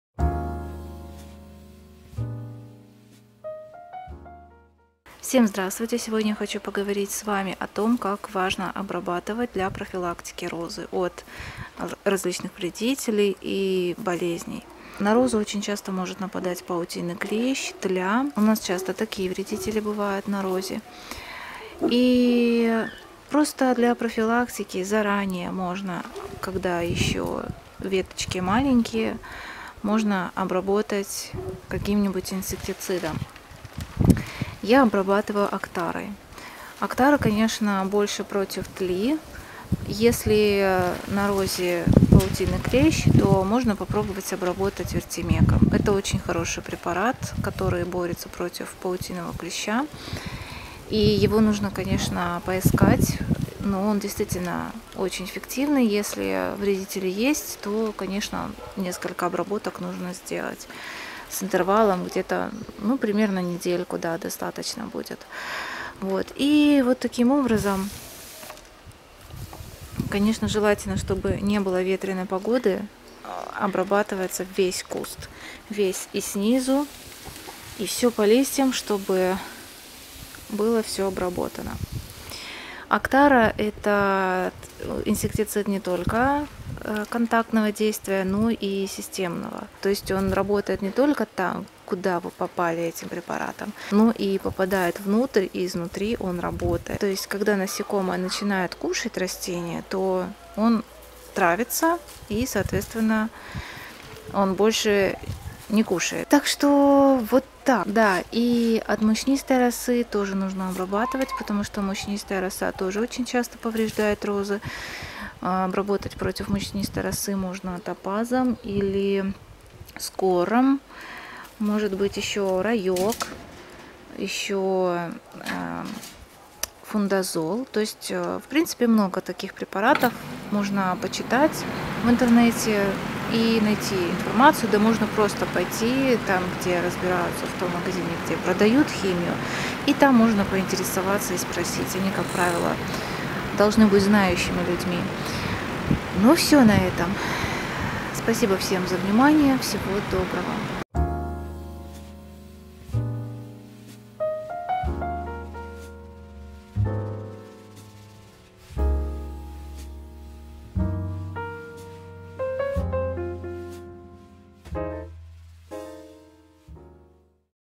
Обработка розы от тли, и от других вредителей и заболеваний. //Валентина Земскова
Всем здравствуйте! Очень важно профилактически обрабатывать розы от различных вредителей и заболеваний.

Все посевы, а также рост многих комнатных растений из семян вы можете проследить в этом плейлисте -